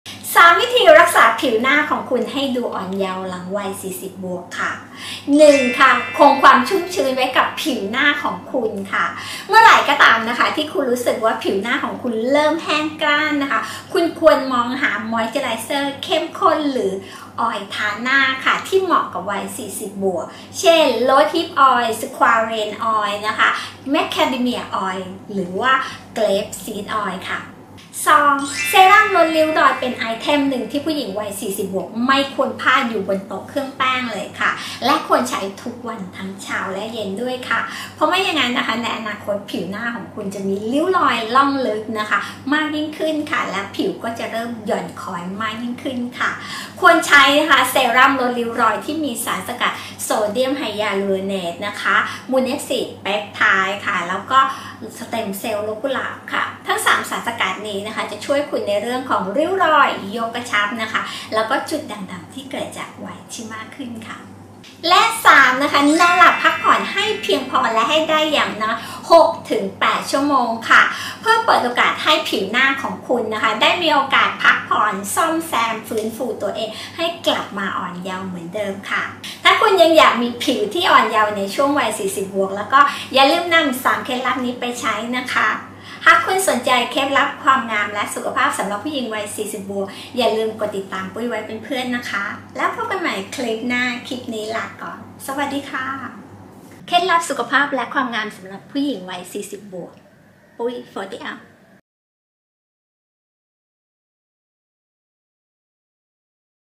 3 วิธีคงความหนุ่มสาวหลังวัย 40+ by คุณแม่วัย 52
3 วิธีคงความหนุ่มสาวหลังวัย 40+
ถ้าสนใจเคล็ดลับสุขภาพและความงามผู้หญิงวัย 40++  หรือทำการตลาดออนไลน์ด้วยวีดีโอคลิก! ติดตามปุ้ยเป็นเพื่อนไว้นะคะ :)
PUI FORTY UP